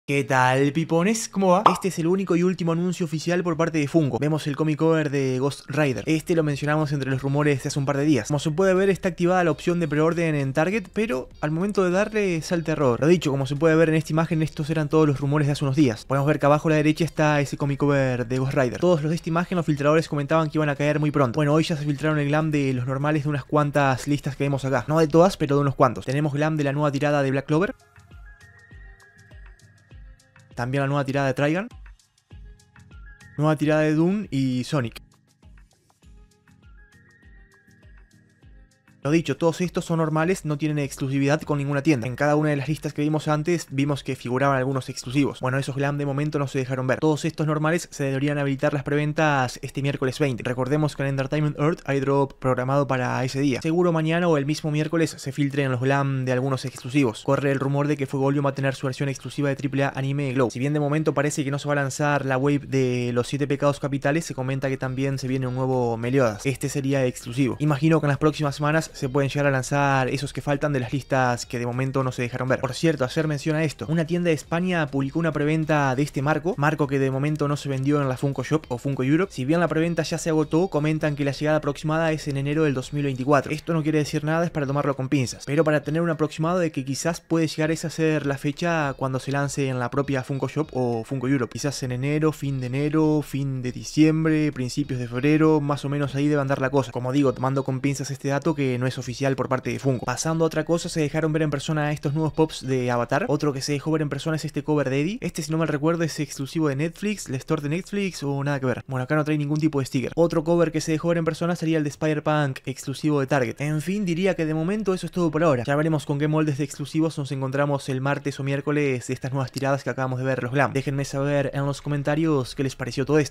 NUEVO FUNKO POP DE METAL SONIC, DR EGGMAN, VASH WITH ANGEL ARM GLOW, ASTA WITH NERO Y MÁS
En este video vemos novedades del mundo Funko POP! a la fecha (18/12/23)
También alguna filtración y vistazo en persona de distintas figuras.